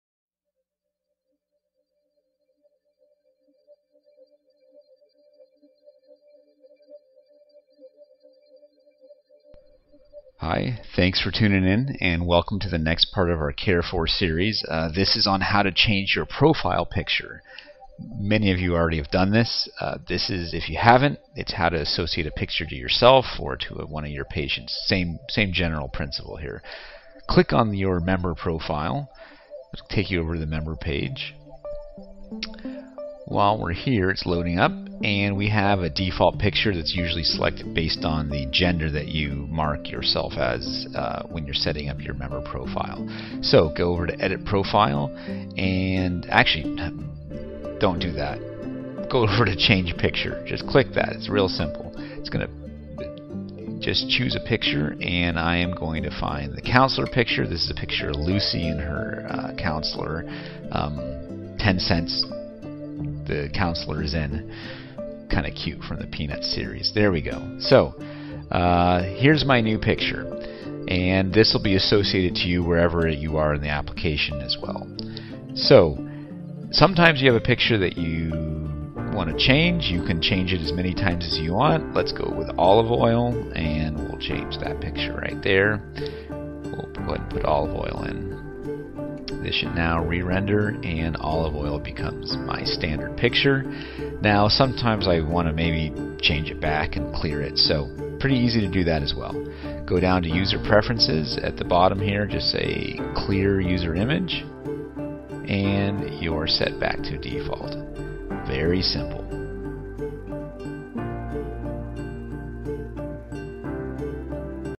Care4Software: Profile Pictures
How To Change or Clear your profile picture